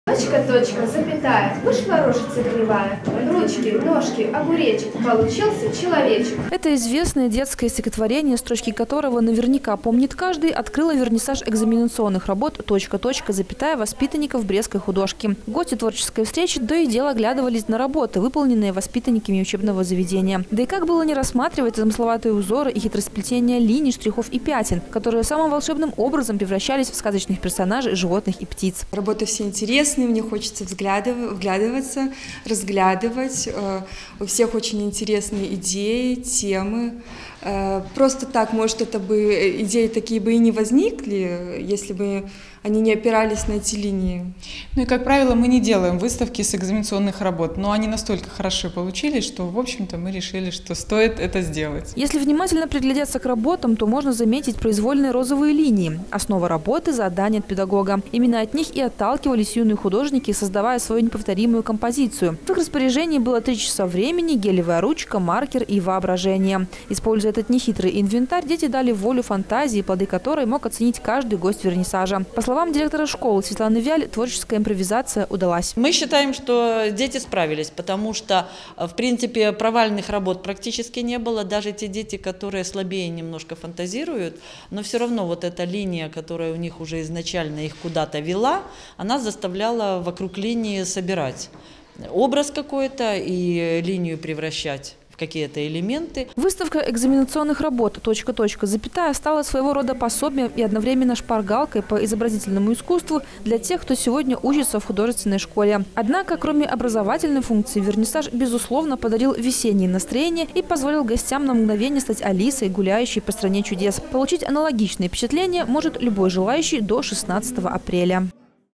2016-04-05 г. Брест. Выставка в Брестской художественной школе. Телекомпания Буг-ТВ.
Точка, точка, запятая. Под таким говорящим названием в Детской школе изобразительных искусств имени Александра Алонцева открылась выставка учебных работ. Герои детских сказок и экзотические животные, необыкновенные пейзажи и натюрморты – всё это многообразие детской фантазии было воссоздано с помощью обычных инструментов - гелевой ручки и маркера. О том, как реализовывались замыслы - узнаем из сюжета